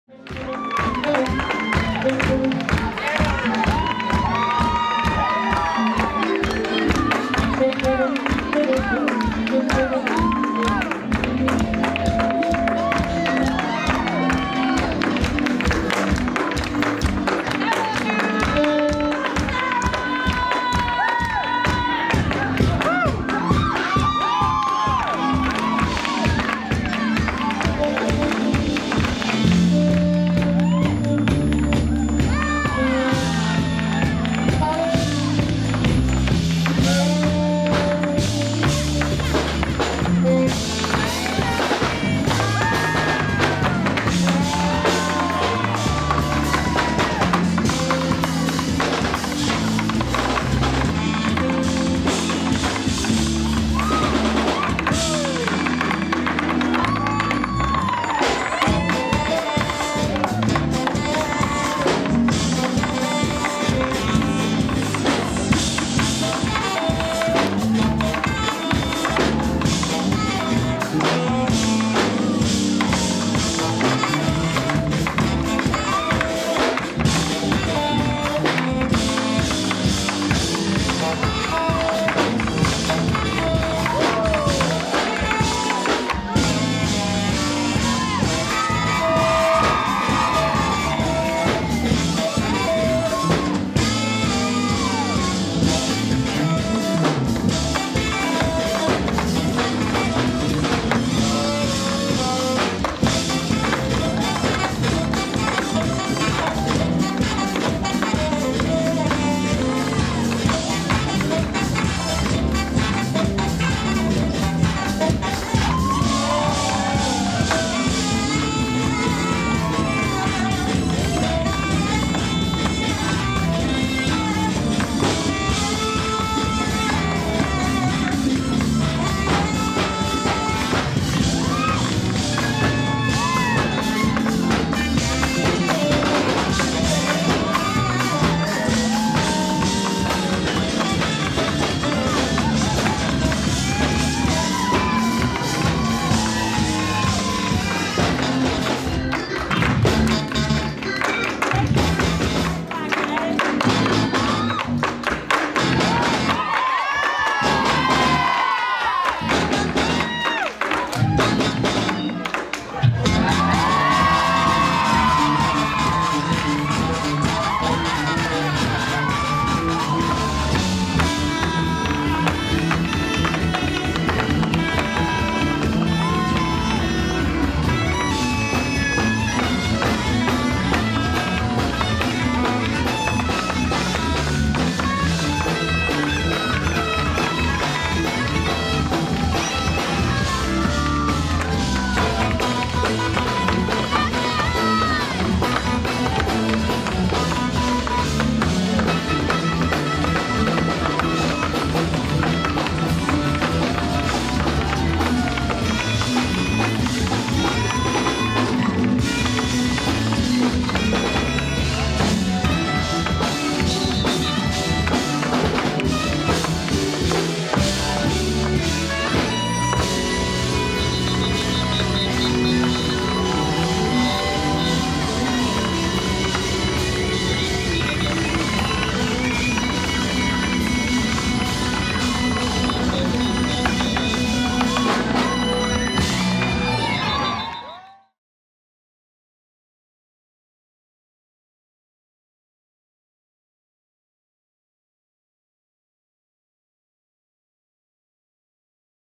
The Floor Improv Night # 38 || Tap Dancing - Sarah Reich & Friends
The Floor # 38 || Tap Dancing with Sarah Reich & Friends || Tap Dancers: Sarah Reich, Chloe Arnold, Kelsey McCowan || Produced by: Carolina Cerisola & Sascha Escandon || Location: King King, Hollywood || May 16, 2011